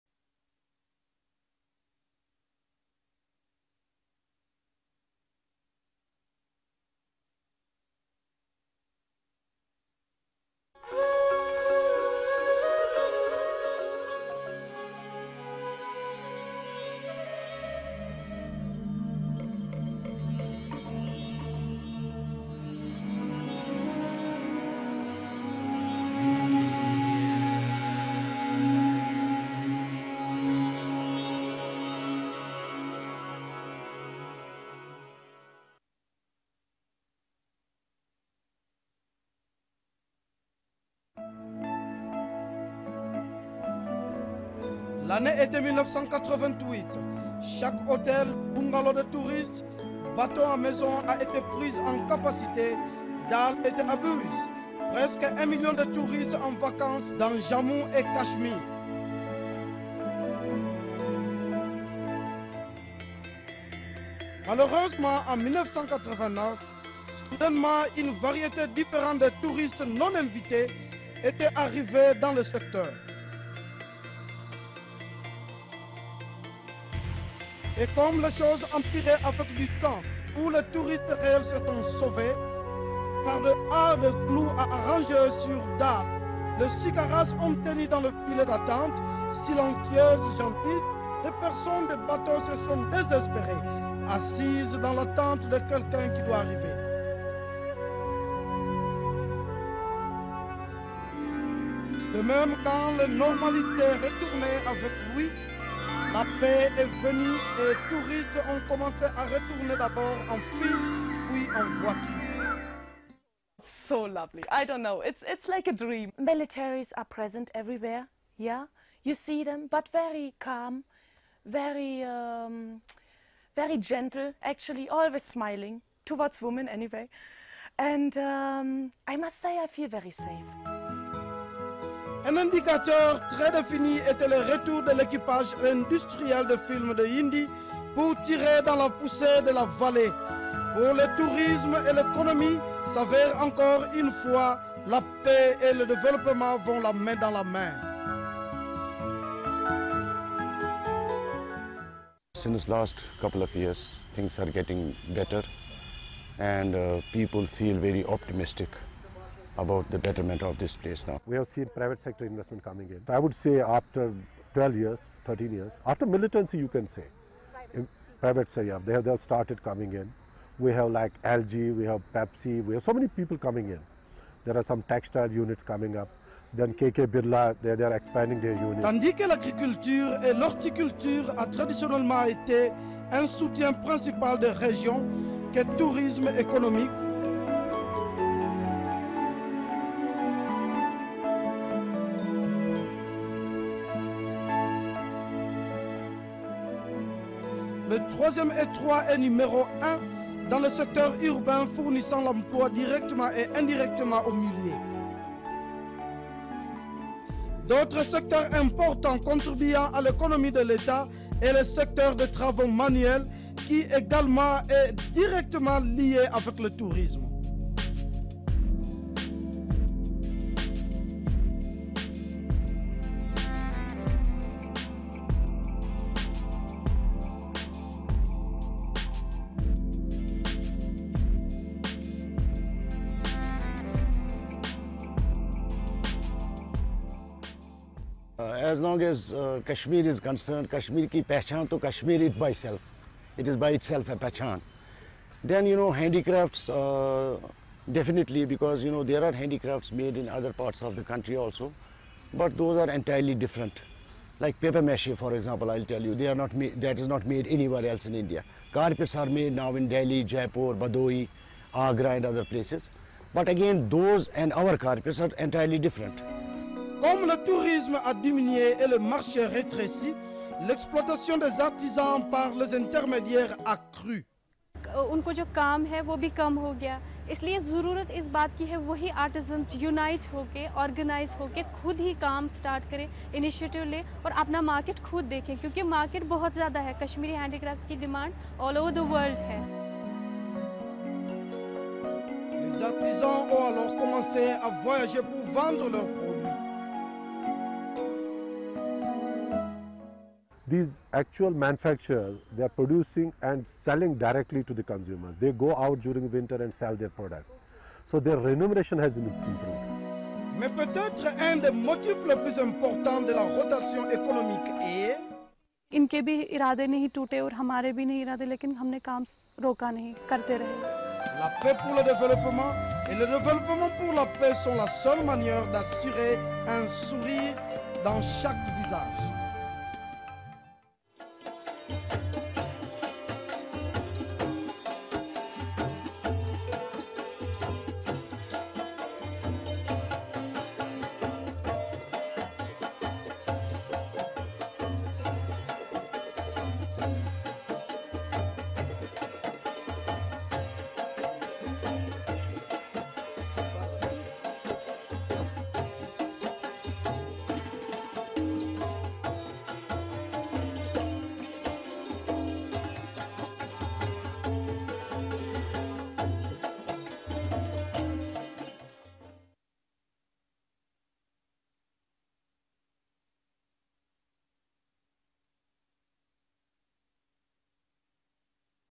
Integral India : Peace for Development (French)
The Setback of Kashmir's tourism industry in the face of the rise of militancy.